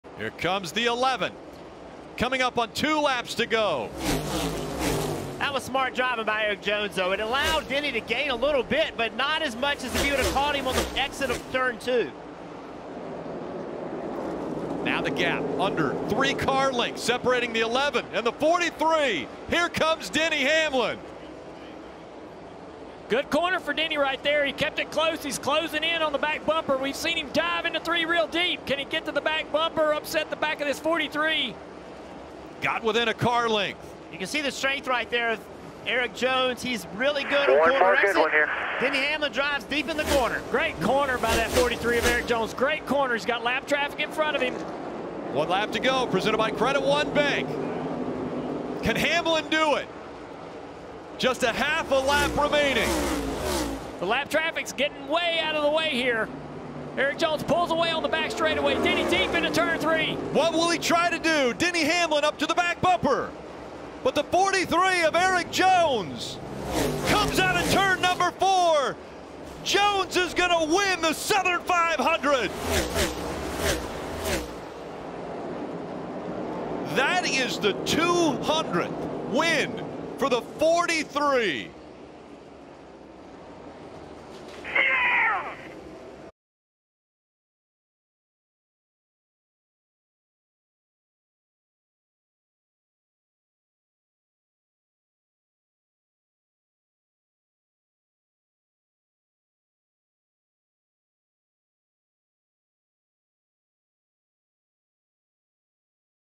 Erik Jones takes Richard Petty to Victory Lane at Darlington Raceway
Watch as Erik Jones holds off Denny Hamlin to win at Darlington taking Richard Petty and the Petty GMS Motorsports' No. 43 to Victory Lane.

Play every weekend!